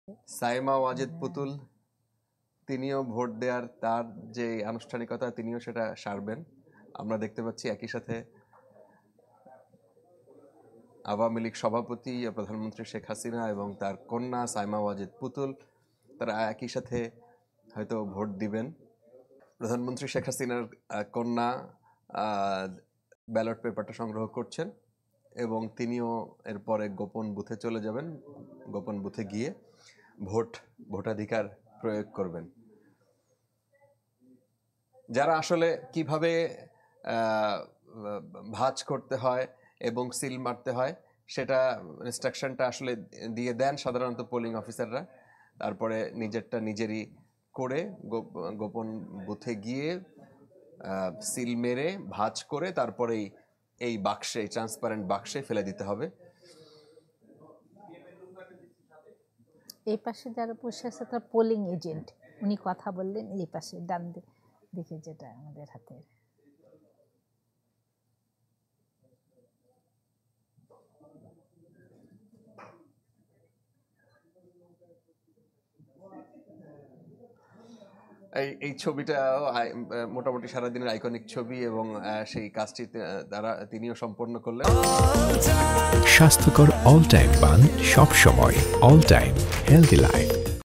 মায়ের পরেই ভোট দিলেন সায়মা ওয়াজেদ পুতুল | Saima Wazed | Election 2024 | Channel 24
মায়ের পরেই ভোট দিলেন সায়মা ওয়াজেদ পুতুল

»» Office Address:
Bangladesh.